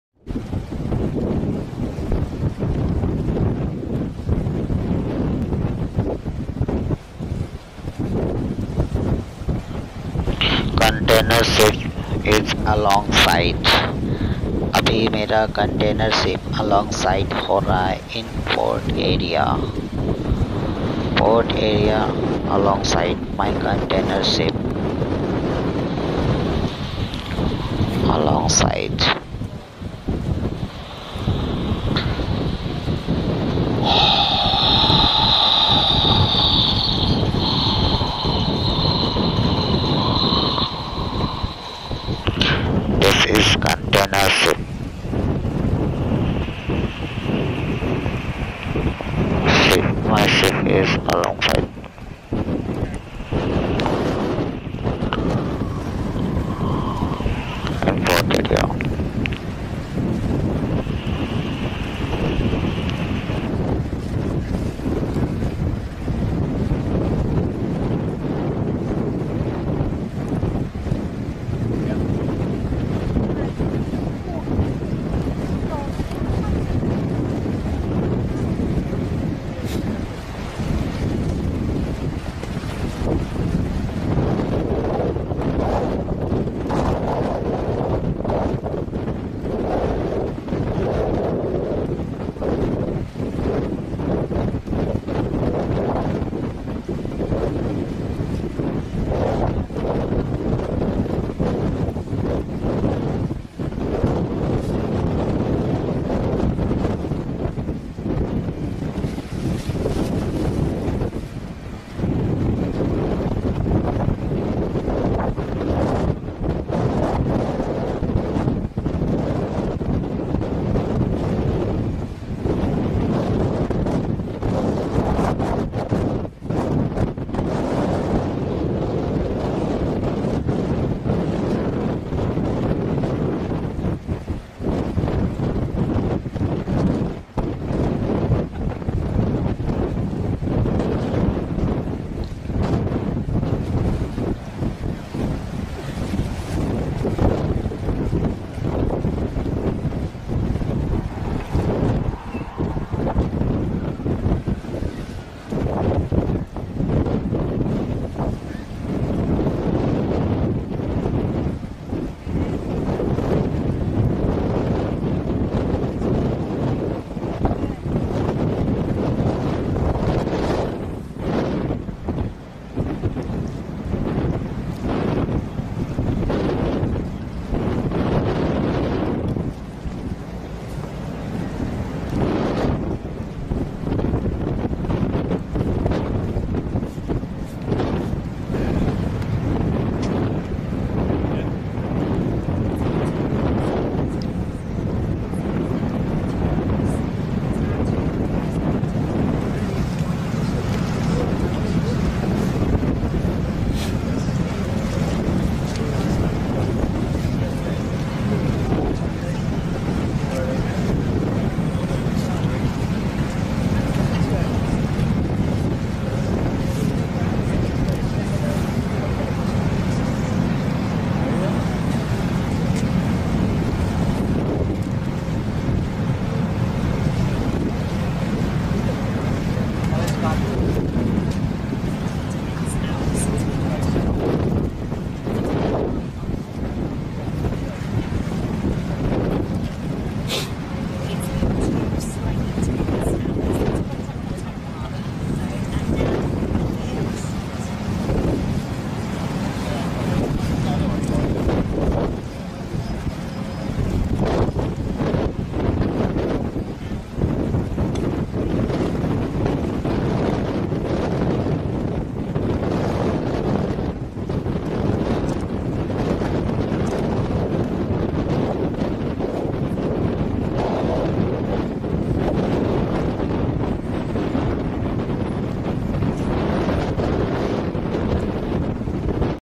Container vessel
Container vessel is along side at container jetty